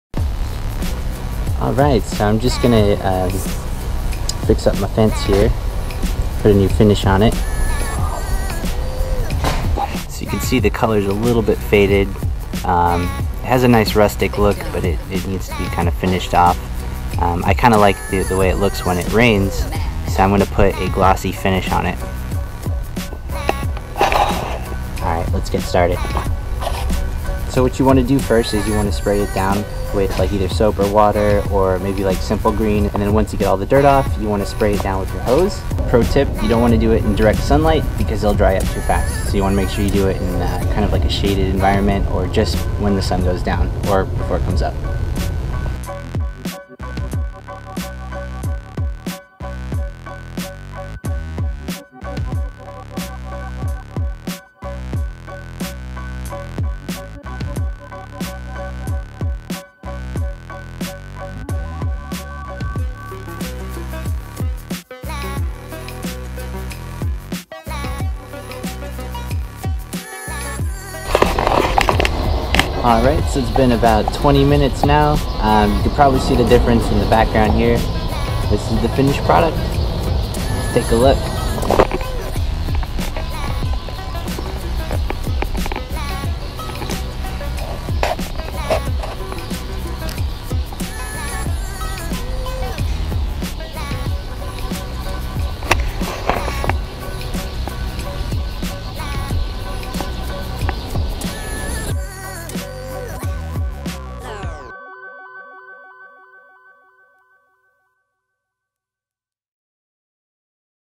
How to refinish a metal gate
I refinished our sliding metal gate with a clear gloss general purpose spray paint from The HomeDepot. I love the way the metal gate looks after it rains so I thought this would be a fun project to get that fresh 'After the Rain' look. Total Cost for project $3.87+tax.